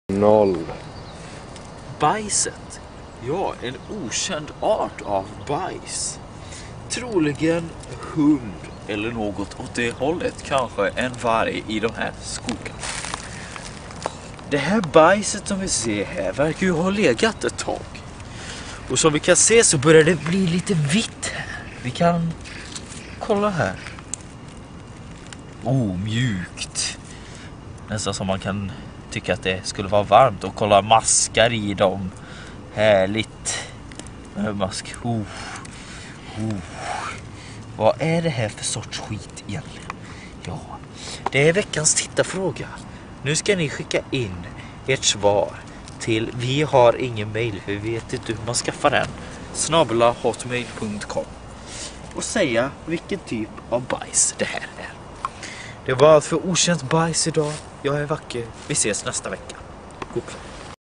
Okänt Bajs, del 1 (Naturprogram)
Del 1 av vårt naturprogram Okänt Bajs. Idén är en parodi på någon "Gissa bajset"-sketch som ingick i något naturprogram för barn runt sent 90-tal.

Denna del och idé spelades in spontant under en paus i samband med inspelningen av vår långfilm "The Shopping Cart Whisperer", någon gång under hösten/vintern 2009.

Bajs är bra.